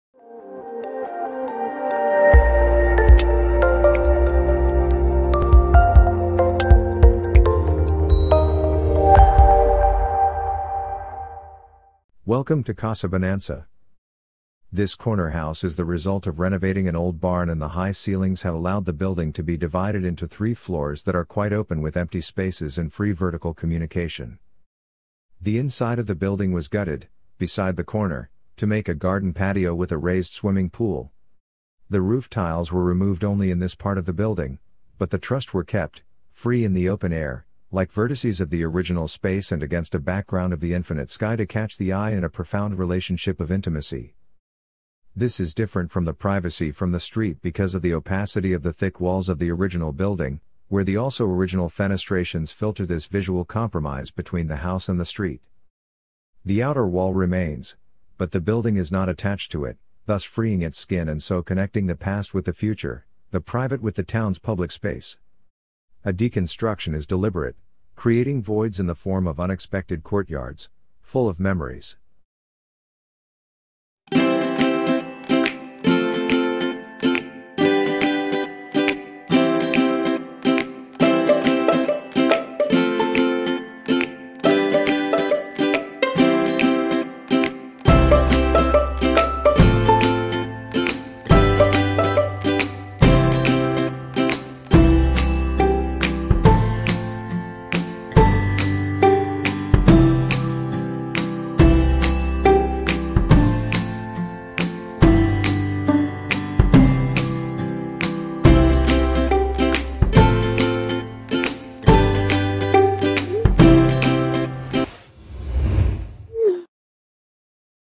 Welcome To Casa Bonança | Mário Martins Atelier
HOUSES•LAGOS, PORTUGAL
Architects: Mário Martins Atelier
Area: 423 m²
Year: 2020
Photographs:Fernando Guerra / FG+SG
Manufacturers: Barbot, Cortizo, Fassa Bortolo, Grohe, Margres,
Saint-Gobain, Sanindusa, Weber, Algarstone, Impermeabilizações, JJ

Teixeira, Jdias
Lead Architect: Mário Martins
Design Team:André Coutinhp, Mariana Franco, Rita Rocha, Sónia

Fialho, José Furtado, Gonçalo Guimarães, So Yeon Lim, Thais

Bressiani, Helder Lima
Structural Engineer / Civil Engineer:Nuno Grave Engenharia
Construction Directors:Marques Antunes Engenharia
City:Lagos
Country:Portugal

This corner house is the result of renovating an old barn and the

high ceilings have allowed the building to be divided into three

floors that are quite open with empty spaces and free vertical

communication.

The inside of the building was gutted, beside the corner, to make a

garden patio with a raised swimming pool. The roof tiles were

removed only in this part of the building, but the trussed were

kept, free in the open air, like vertices of the original space and

against a background of the infinite sky to catch the eye in a

profound relationship of intimacy.

This is different from the privacy from the street because of the

opacity of the thick walls of the original building, where the also

original fenestrations filter this visual compromise between the

house and the street.

The outer wall remains, but the building is not attached to it, thus

freeing its “skin” and so connecting the past with the future, the

private with the town’s public space. A “deconstruction” is

deliberate, creating voids in the form of unexpected courtyards,

full of memories.